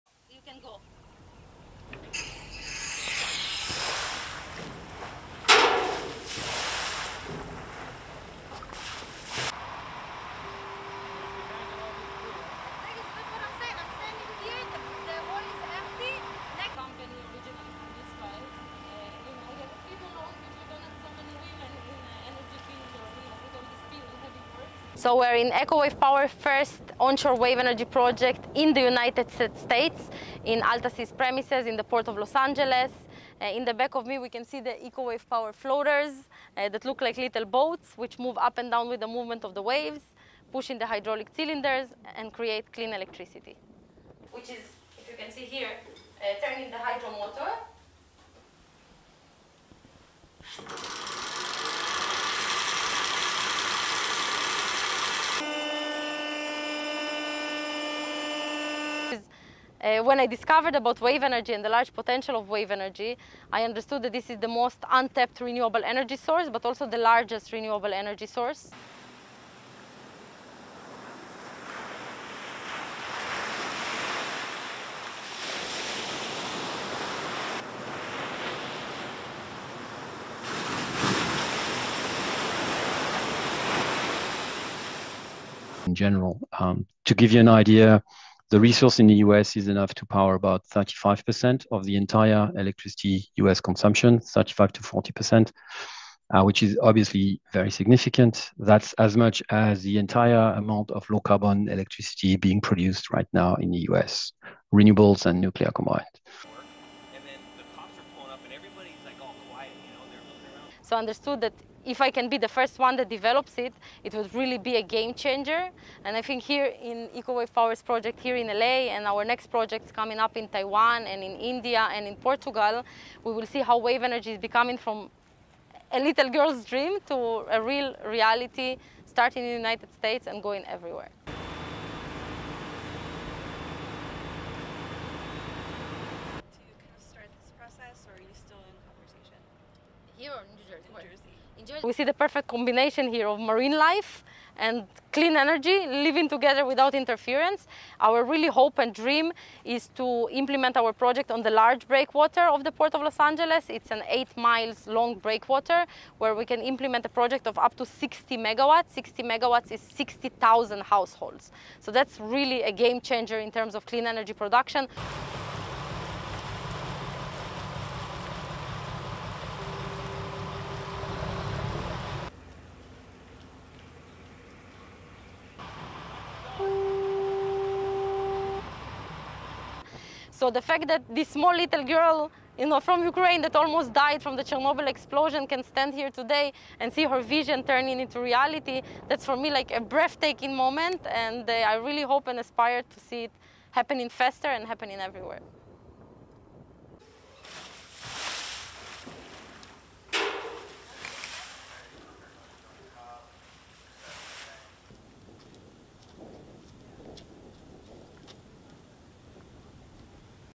First ever US wave energy project is set to launch
RESTRICTION SUMMARY:

ASSOCIATED PRESS
Los Angeles - 4 September 2025
1.  Floater devices descend into the water at the Eco Wave Power onshore wave energy site
2. Inna Braverman, Eco Wave Power co-founder and CEO looks out from a boat at the port of Los Angeles breakwater
3. Floater devices bob in the waves as they generate power
4. SOUNDBITE (English) Inna Braverman, co-founder and CEO of Eco Wave Power:
"So we're in Eco Wave Power's first onshore wave energy project in the United States in AltaSea's premises in the port of Los Angeles. In the back of me we can see the Eco Wave Power floaters that look like little boats which move up and down with the movement of the waves, pushing the hydraulic cylinders and create clean electricity."
5. Braverman points out the wave generating technology
6. Close up of the hydraulic energy system
7. SOUNDBITE (English) Inna Braverman, co-founder and CEO of Eco Wave Power:
"When I discovered about wave energy and the large potential of wave energy, I understood that this is the most untapped renewable energy source but also the largest renewable energy source."

ASSOCIATED PRESS
Newport Beach, California - 5 September 2025
8. Various of waves hitting a jetty and the shoreline

ASSOCIATED PRESS
Brussels - 5 September 2025
9. SOUNDBITE (English) Rémi Gruet, CEO of Ocean Energy Europe:
"To give you an idea, the resource in the US is enough to power about 35% of the entire electricity US consumption, 35 to 40%, which is obviously very significant. That's as much as the entire amount of low carbon electricity being produced right now in the US, renewables and nuclear combined."

ASSOCIATED PRESS
Los Angeles - 04 September 2025
10. A floater is shown with a boat passing by

ASSOCIATED PRESS
11. SOUNDBITE (English) Inna Braverman, co-founder and CEO of Eco Wave Power:
"So I understood that if I can be the first one that develops it, it will really be a game changer. And I think here in Eco Wave Power's project here in LA and our next project is coming up in Taiwan and in India and in Portugal, we will see how wave energy is becoming from a little girl's dream to a real reality starting in the United States and going everywhere."
12. View of building at the port of Los Angeles
13. A bird swims in the water near the floaters
14. SOUNDBITE (English) Inna Braverman, co-founder and CEO of Eco Wave Power:
"We see the perfect combination here of marine life and clean energy living together without interference. Our really hope and dream is to implement our project on the large breakwater of the Port of Los Angeles. It's an eight miles long breakwater where we can implement a project of up to 60 megawatts. 60 megawatts is 60,000 households. So that's really a game changer in terms of clean energy production."
15. A view of the breakwater
16. A boat on the water
17. A lighthouse sounds its horn
18. SOUNDBITE (English) Inna Braverman, co-founder and CEO of Eco Wave Power:
"So the fact that this small little girl from Ukraine that almost died from the Chernobyl explosion can stand here today and see her vision turning into reality, that's for me like a breathtaking moment. And I really hope and aspire to see it happening faster and happening everywhere."
19. Various of the floaters

STORYLINE:
At the port of Los Angeles, on a site that once housed oil tanks, seven steel structures that look like small blue boats are lowered into the ocean, where they gently bob up and down with the waves’ motions to generate power.